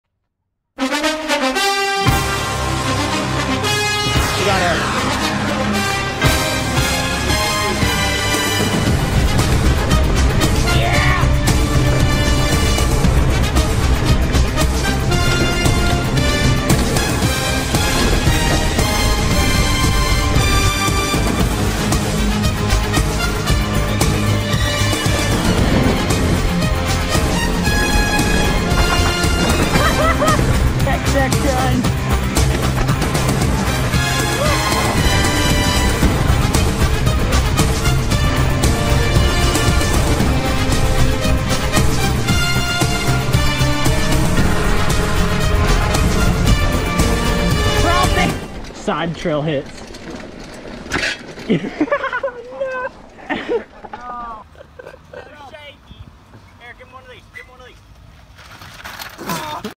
$25 Goodwill Bike at Spider Mountain
A quick fun edit from this past weekend at Spider Mountain.

Camera: GoPro Hero9 Black (Chest mounted)
Bike: Pivot Mach 6 2021
Elbow Pads: Kali Protectives Strike
Knee Pads: Kali Protectives Strike
Ass and Thigh Pads: BARHAR Protective Armor Pants
Chest Protector: Fox Race Subframe
Neck Brace: Leatt DBX 3.5
Helmet: Fox Rampage